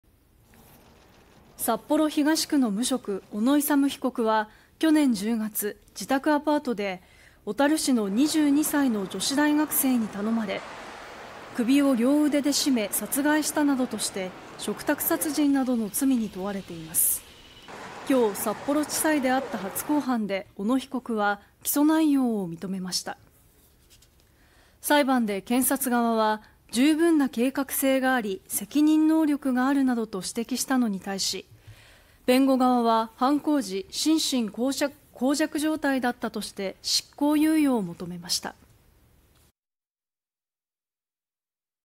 女子大学生 "嘱託殺人" 被告の男（54） 起訴内容認める… 【悩みを抱えたら相談を】 (23/08/31 11:55)
女子大学生に依頼され首を絞め殺害した罪などに問われている男の初公判が札幌地裁で開かれ、男は起訴内容を認めました。

　札幌市東区の無職 小野勇被告（54）は、2022年10月に自宅アパートで北海道小樽市の22歳の女子大学生に頼まれ、首を両腕で絞めて殺害したうえ遺体の左脇の部分を刃物で切り裂いたとして、嘱託殺人などの罪に問われています。

　8月31日の札幌地裁での初公判で、小野被告は裁判官から「起訴事実に間違っていることはありますか？」と問われ「いえ、ありません」と答え、起訴内容を認めました。

　裁判では検察側が「十分な計画性があり責任能力がある」などと指摘したのに対し、弁護側は「犯行時に心神耗弱状態だった」として執行猶予を求めました。

　【悩みを抱えたら相談を】

　北海道いのちの電話

　札幌市若年女性支援事業LiNK
　LINE：＠764nqgbc